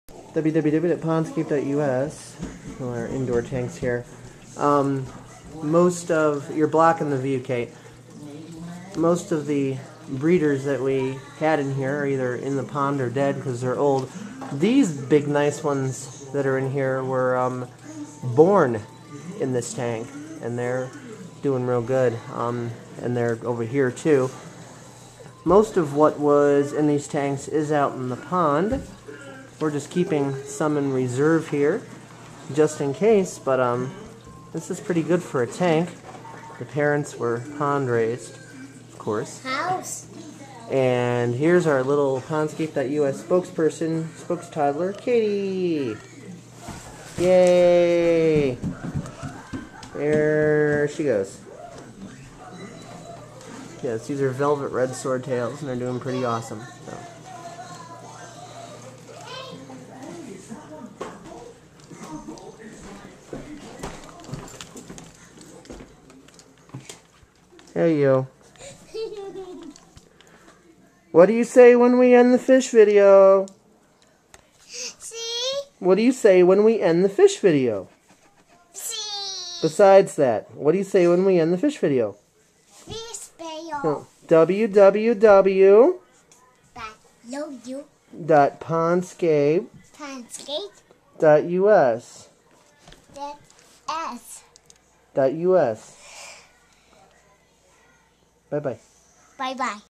Pondscape.us - July-07 - 2009 - Velvet Red Sword Tails (Breeding Update)
Velvet Red Swordtails are TROPICAL FRESH WATER FISH (Xiphophorus hellerii in family Poeciliidae of order Cyprinodontiformes). A live-bearer, it is closely related to the southern platyfish or "platy" (X. maculatus) and can interbreed with it. It is native to an area of North and Central America stretching from Veracruz, Mexico, to northwestern Honduras.